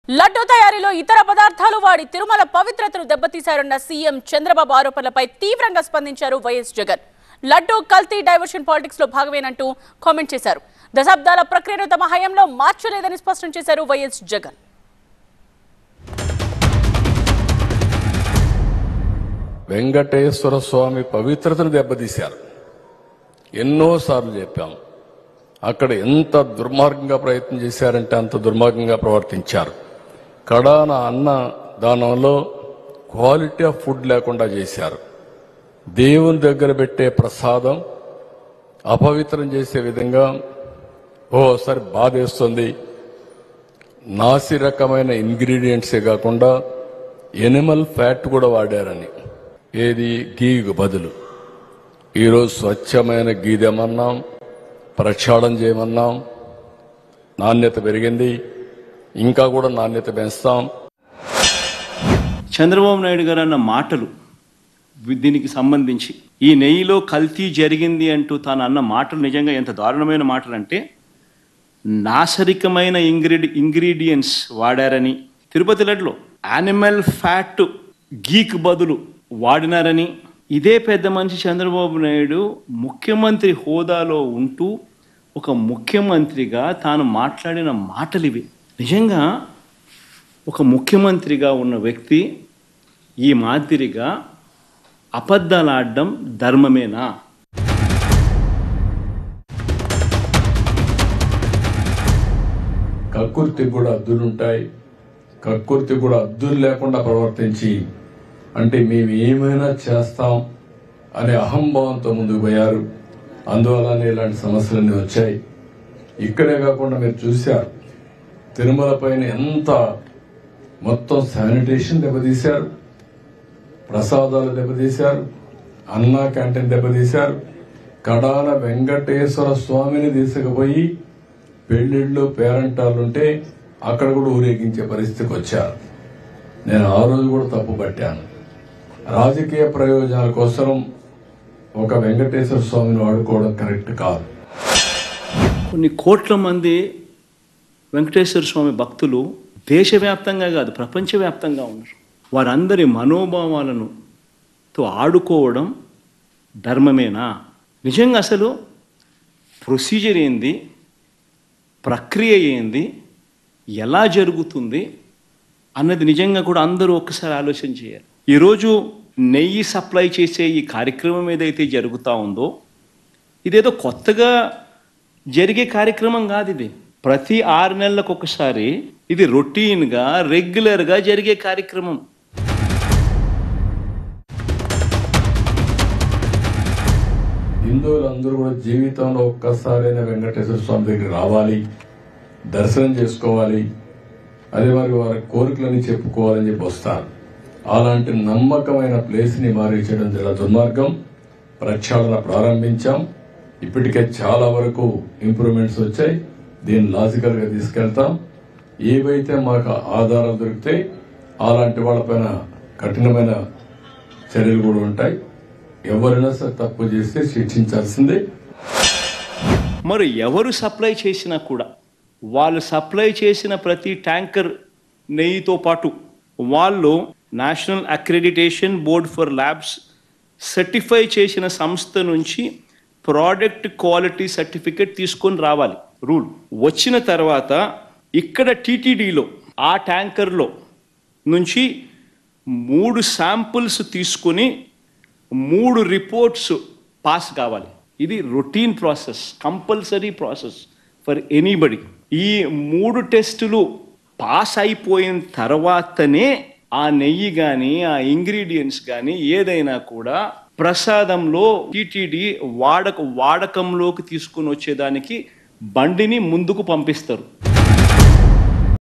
Dailouge War : CM Chandrababu Vs YS Jagan - TV9
Credits